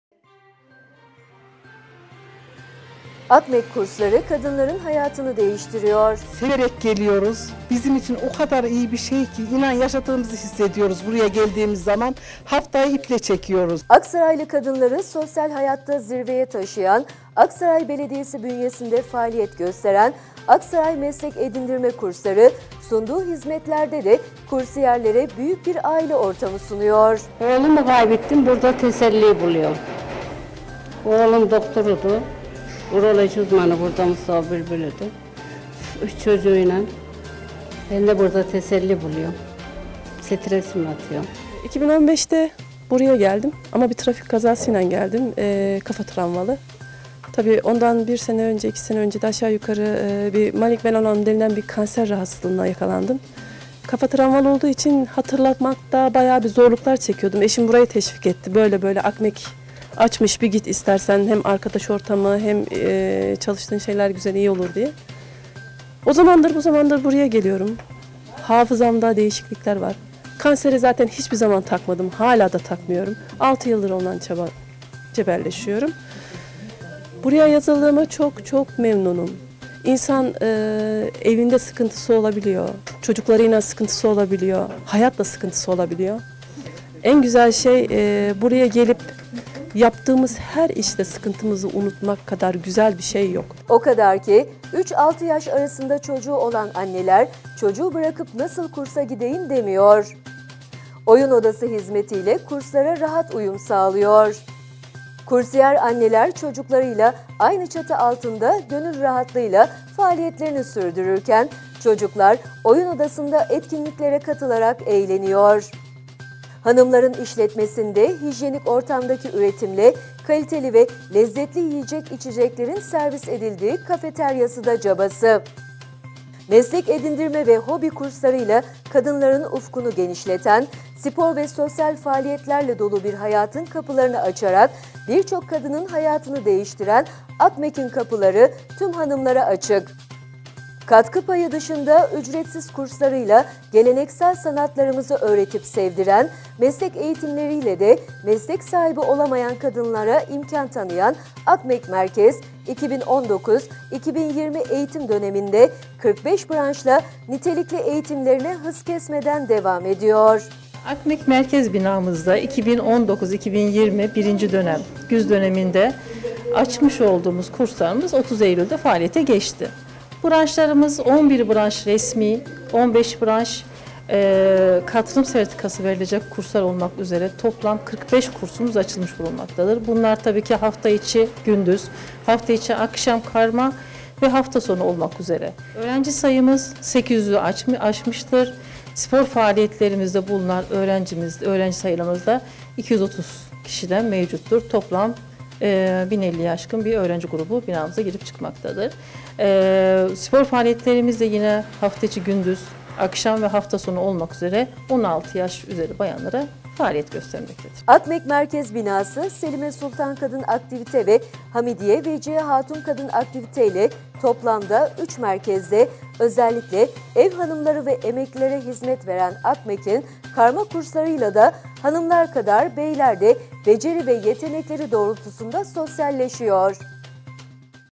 AKMEK KURSLARI KADINLARIN HAYATINI DEĞİŞTİRİYOR
AKSARAYLI KADINLARI SOSYAL HAYATTA ZİRVEYE TAŞIYAN AKSARAY BELEDİYESİ BÜNYESİNDE FAALİYET GÖSTEREN AKSARAY MESLEK EDİNDİRME (AKMEK) KURSLARI SUNDUĞU HİZMETLERDE DE KURSİYERLERE BÜYÜK BİR AİLE ORTAMI SUNUYOR.
O KADAR Kİ 3-6 YAŞ ARASINDA ÇOCUĞU OLAN ANNELER “ÇOCUĞU BIRAKIP NASIL KURSA GİDEYİM” DEMİYOR. OYUN ODASI HİZMETİ İLE KURSLARA RAHAT UYUM SAĞLIYOR.
KURSİYER ANNELER ÇOCUKLARI İLE AYNI ÇATI ALTINDA GÖNÜL RAHATLIĞI İLE   FAALİYETLERİNİ SÜRDÜRÜRKEN ÇOCUKLAR OYUN ODASINDA ETKİNLİKLERE KATILARAK EĞLENİYOR.
HANIMLARIN İŞLETMESİNDE HİJYENİK ORTAMDAKİ ÜRETİMLE KALİTELİ VE LEZZETLİ YİYECEK-İÇECEKLERİN SERVİS EDİLDİĞİ KAFETERYASIDA CABASI.
MESLEK EDİNDİRME VE HOBİ KURSLARIYLA KADINLARIN UFKUNU GENİŞLETEN, SPOR VE SOSYAL FAALİYETLERLE DOLU BİR HAYATIN KAPILARINI AÇARAK BİRÇOK KADININ HAYATINI DEĞİŞTİREN AKMEK’İN KAPILARI TÜM HANIMLARA AÇIK.
KATKI PAYI DIŞINDA ÜCRETSİZ KURSLARIYLA GELENEKSEL SANATLARIMIZI ÖĞRETİP SEVDİREN, MESLEK EĞİTİMLERİ İLE DE MESLEK SAHİBİ OLAMAYAN KADINLARA İMKÂN TANIYAN AKMEK MERKEZ, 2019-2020 EĞİTİM DÖNEMİNDE  45 BRANŞLA NİTELİKLİ EĞİTİMLERİNE HIZ KESMEDEN DEVAM EDİYOR.
AKMEK MERKEZ BİNASI, SELİME SULTAN KADIN AKTİVİTE VE HAMİDİYE VECİYE HATUN KADIN AKTİVİTE İLE TOPLAMDA 3 MERKEZDE ÖZELLİKLE EV HANIMLARI VE EMEKLİLERE HİZMET VEREN AKMEK’İN KARMA KURSLARI İLE HANIMLAR KADAR BEYLERDE BECERİ VE YETENEKLERİ DOĞRULTUSUNDA SOSYALLEŞİYOR.